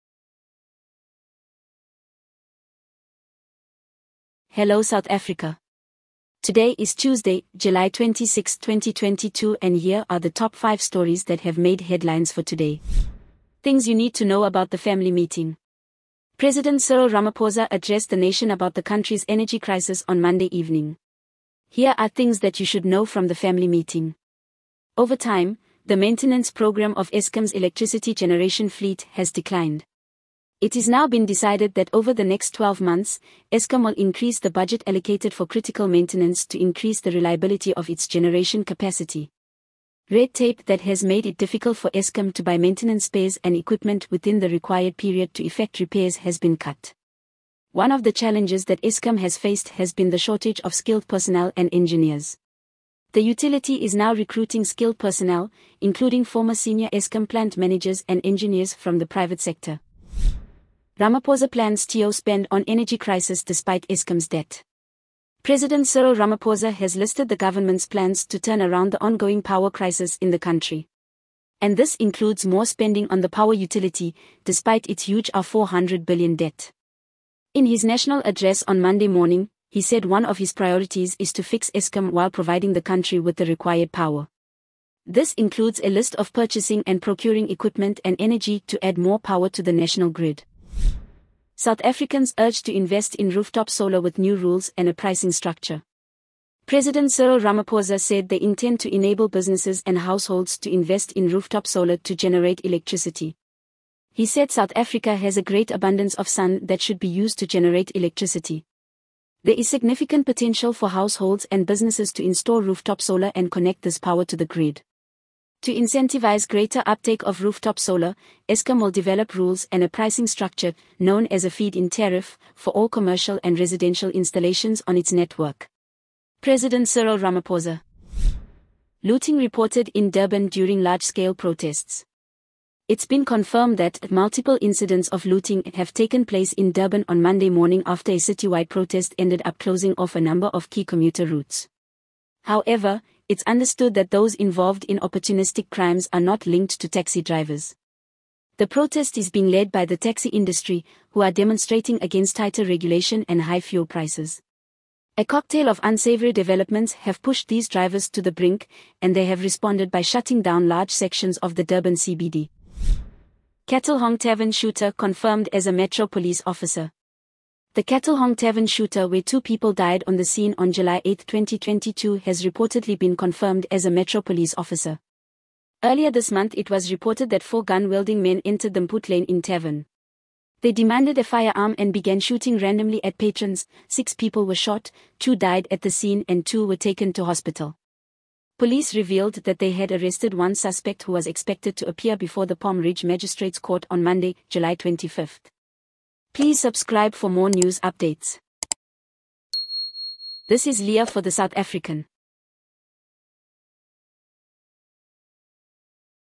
DAILY NEWS BRIEFING: Things you need to know about the family meeting
President Cyril Ramaphosa addressed the nation about the country’s energy crisis on Monday evening. Here are things that you should know from the family meeting.

Videos: Pixabay, Twitter/Abramjee, Twitter/SAPS, Twitter/PresidencyZA